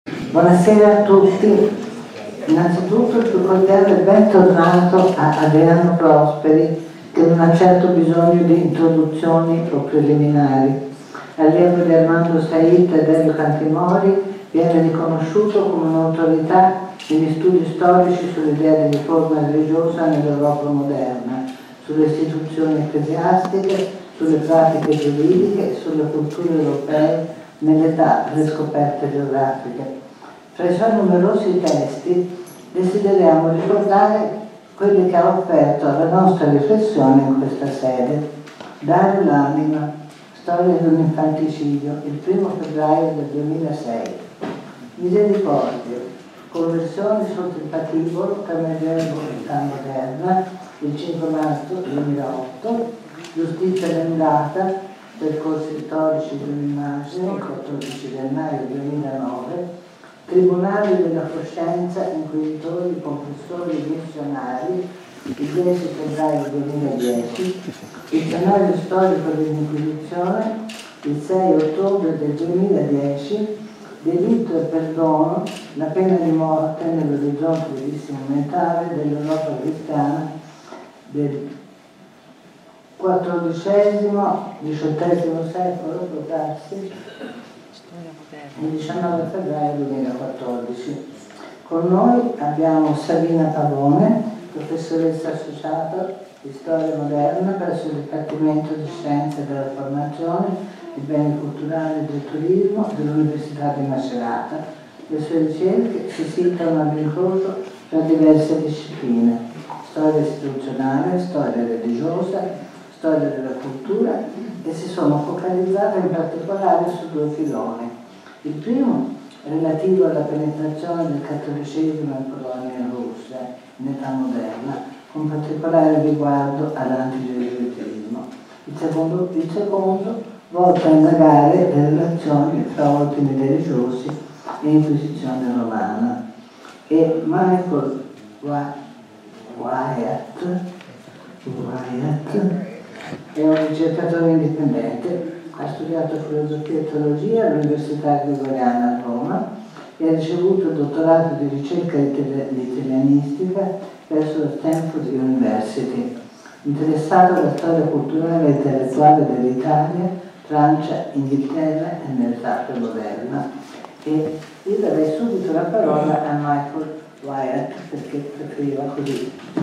XXI LPND Prosperi 1 di 5
Leggere per non dimenticare XXI Stagione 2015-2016
Ciclo di incontri ideato e curato da Anna Benedetti per la Regione Toscana e il Comune di Firenze
Presentazione del libro "La vocazione. Storie di gesuiti tra Cinquecento e Seicento" di Adriano Prosperi
Introduzione di Sabina Pavone e Michael Wyatt
Biblioteca delle Oblate - Firenze - 11/05/2016